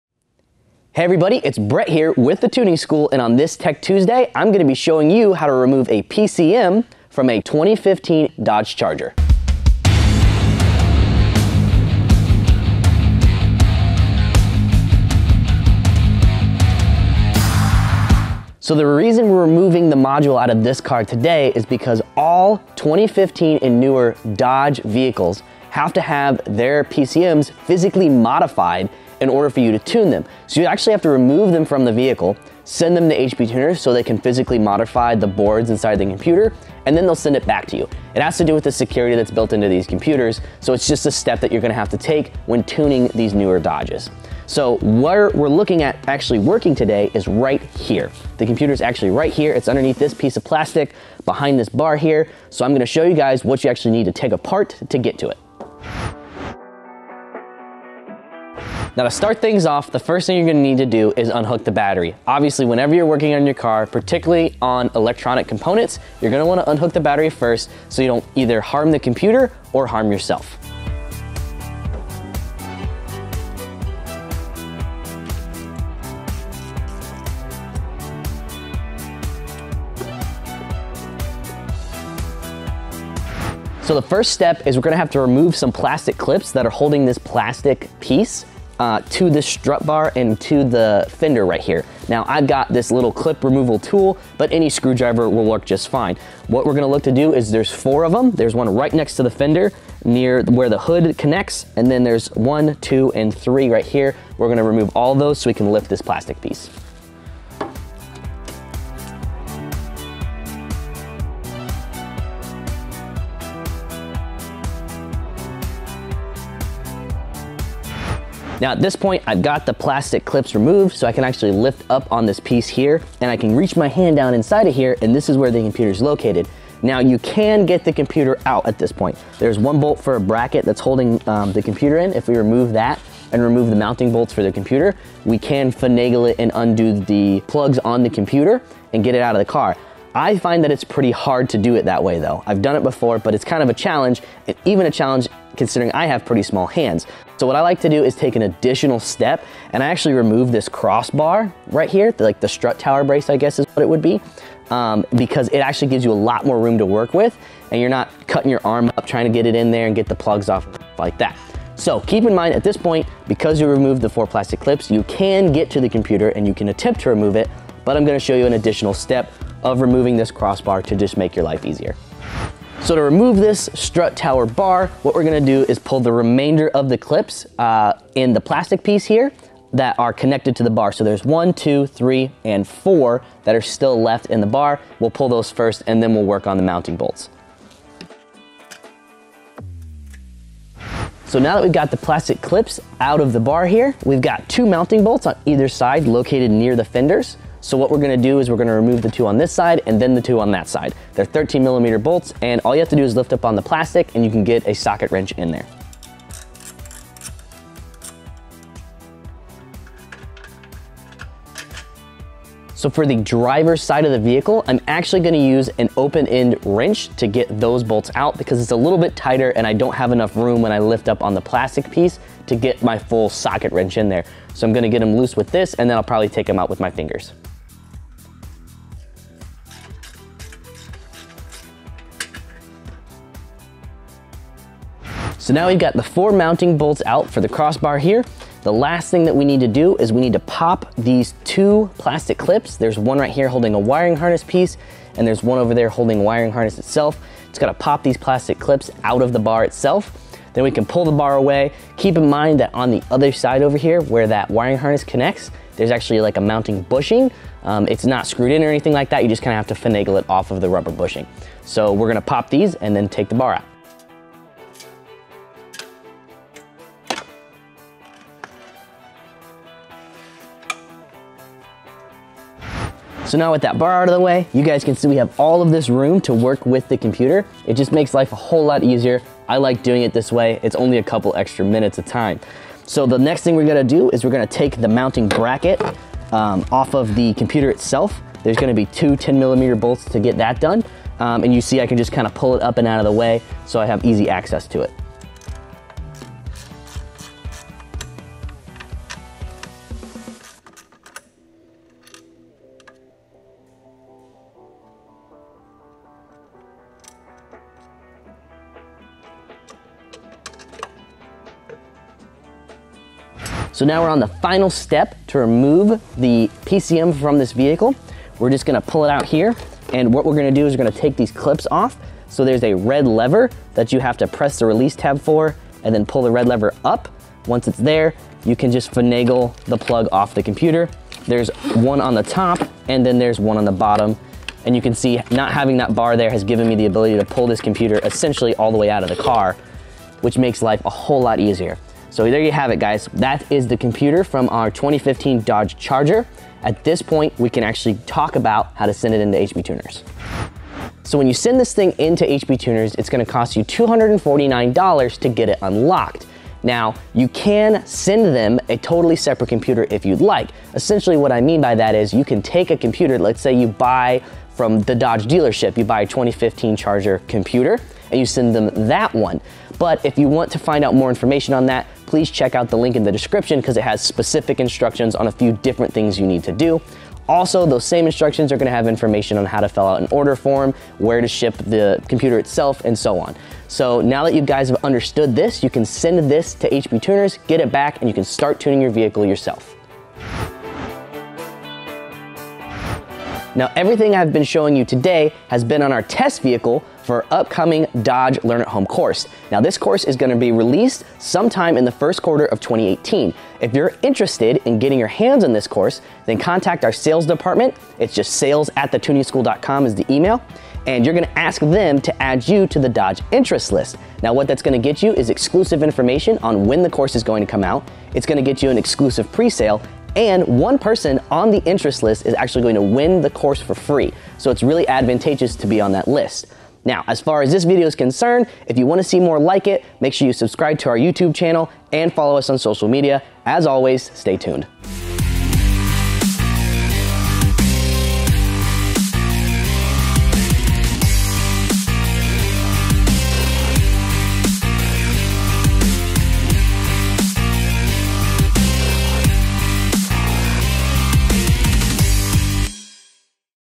How to Physically Remove Dodge PCM Before Tuning 2015 and Newer Using HP Tuners
This video will show you step by step how to remove a Dodge PCM and send it into HP Tuners for their modification service.